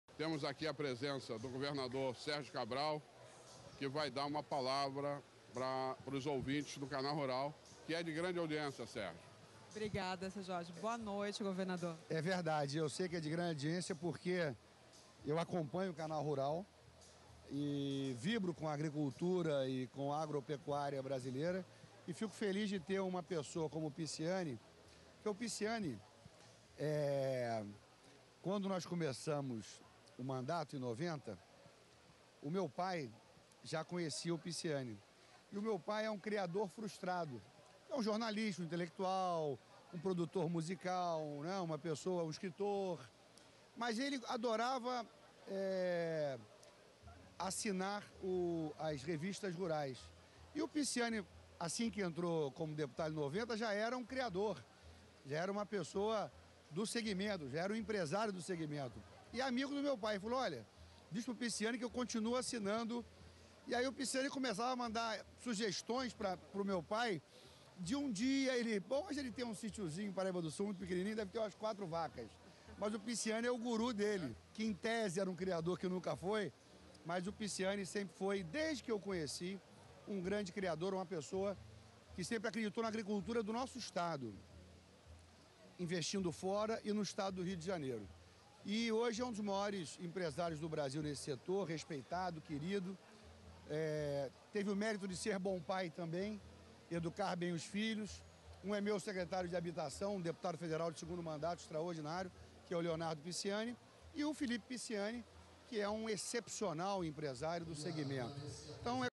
Governador Sérgio Cabral fala sobre a importância do empresário Picciani
Governador Sérgio Cabral fala a importância do empresário Picciani